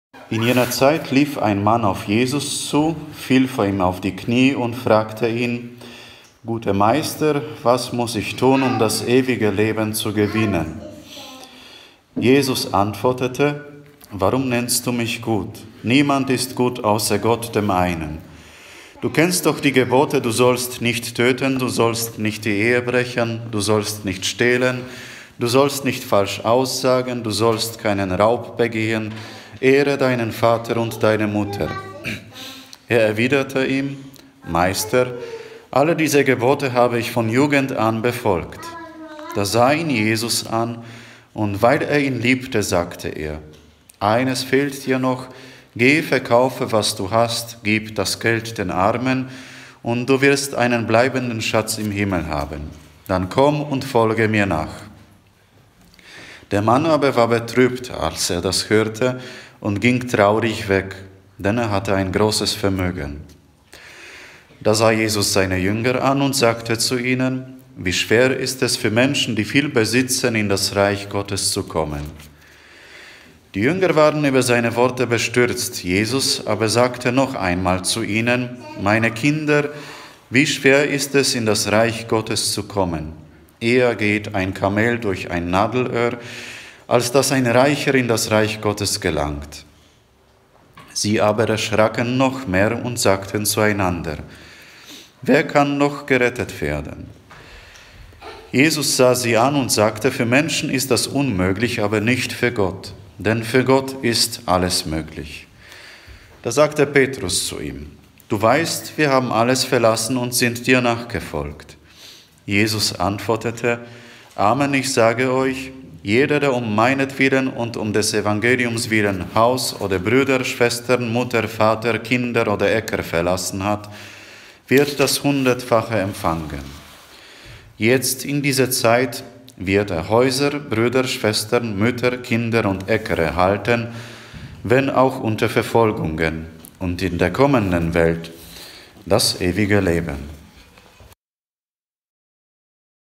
28. Sonntag im Jahreskreis B - Evangelium (Mk 10, 17-30)
Evangelium zum 28. Sonntag im Jahreskreis B (Mk 10, 17-30)